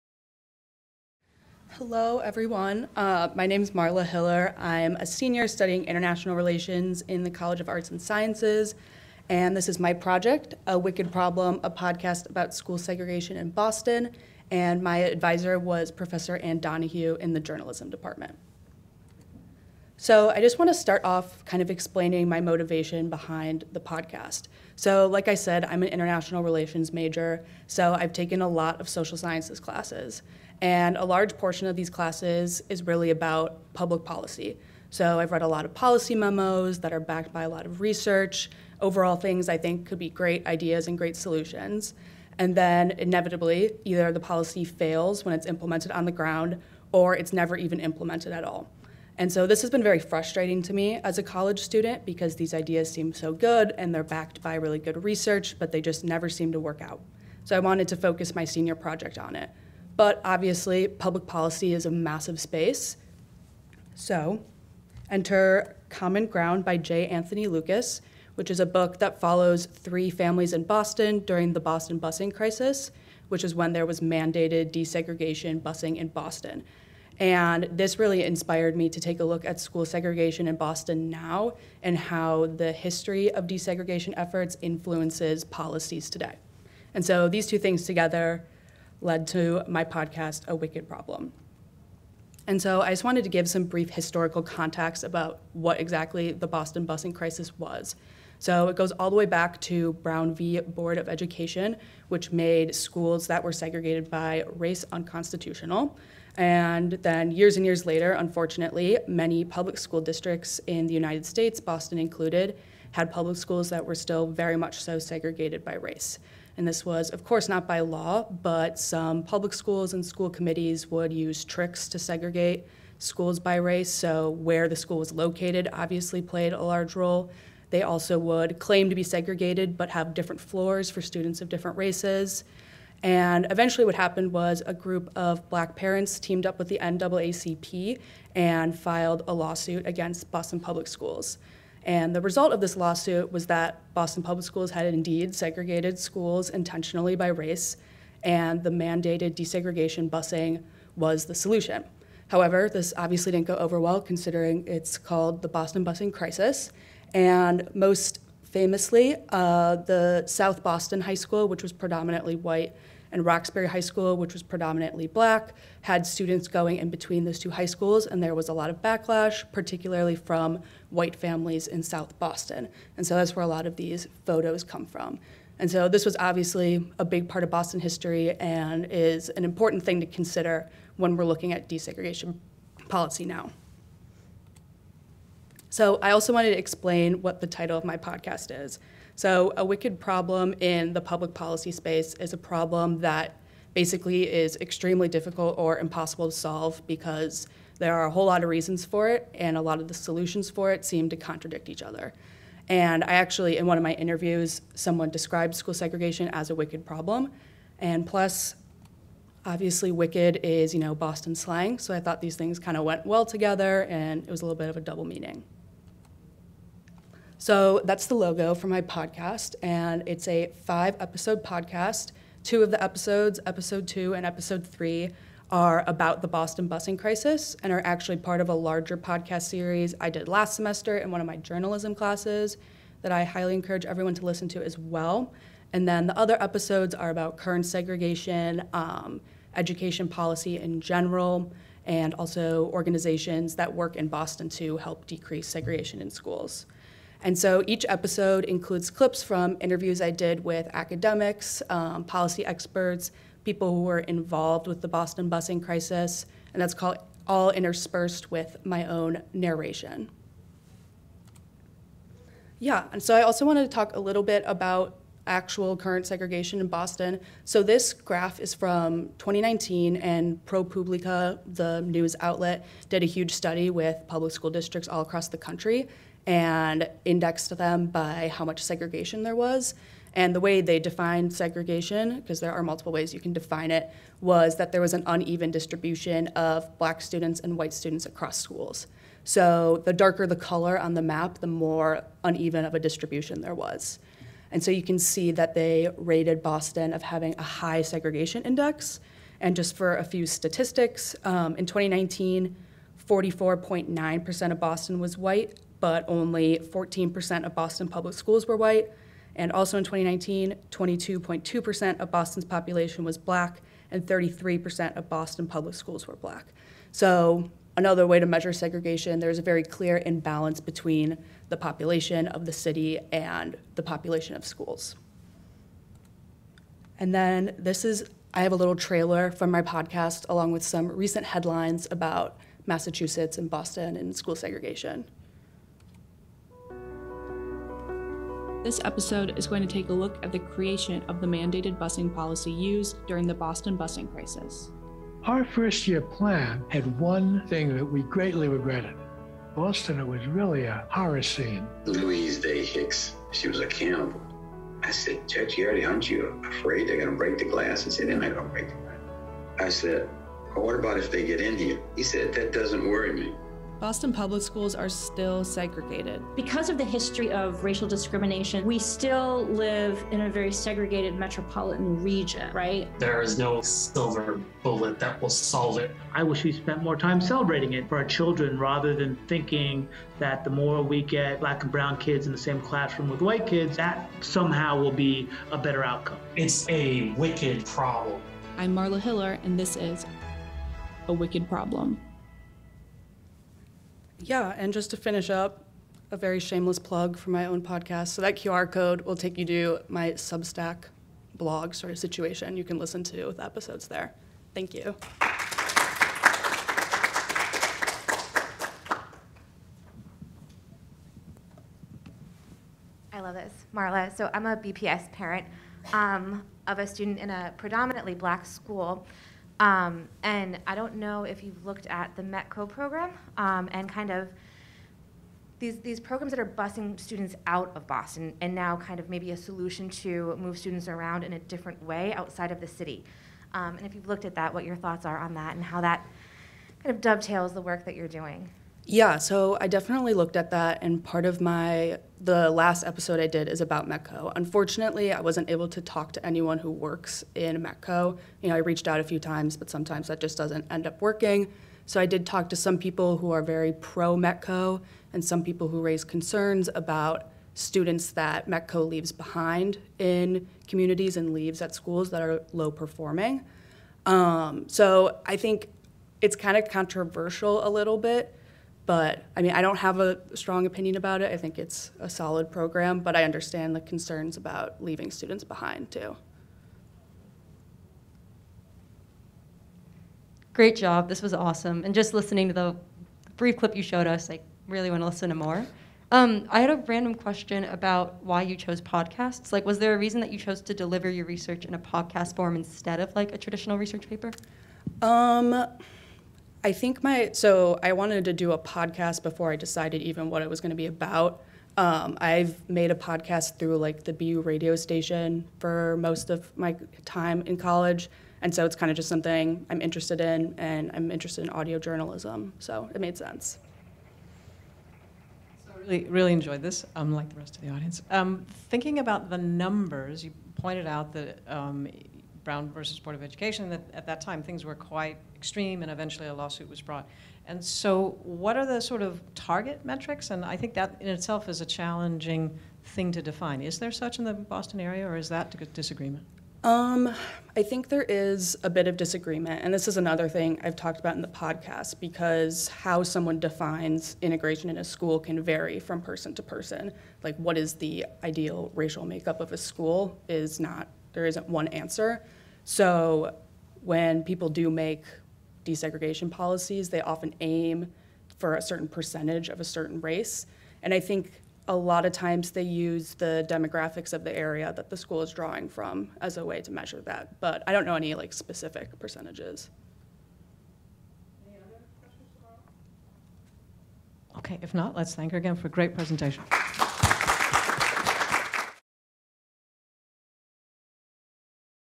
Marla Hiller, 2022 Keystone Presentation
Marla Hiller - A Wicked Problem: A Podcast About School Segregation in Boston

9th Annual Kilachand Honors College Keystone Symposium: Together Again, United By Exploration